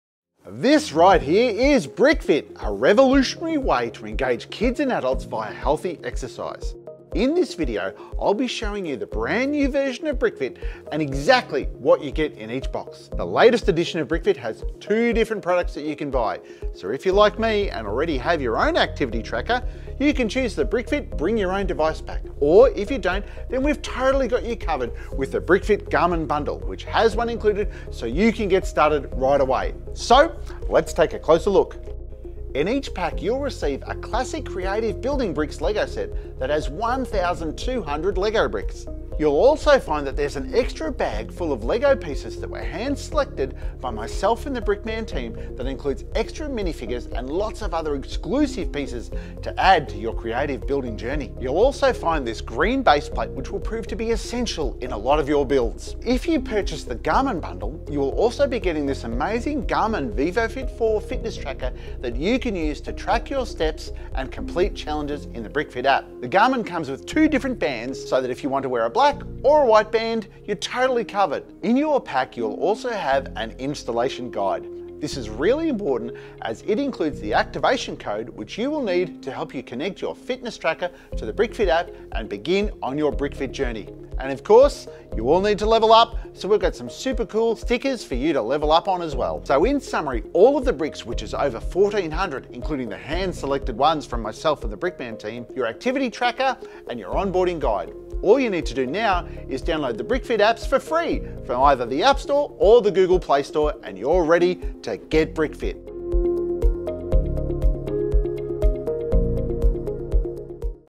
NEW BrickFit Unboxing!
Excited to see what you get with the new BrickFit? 😃 Watch as Brickman takes us through everything in the box!

🏃Whether you have your own activity tracker🏃‍♀️, or are needing one, BrickFit has you covered with our BYO Device Bundle and our Garmin Bundle which includes a Garmin Vivofit 4! 🎉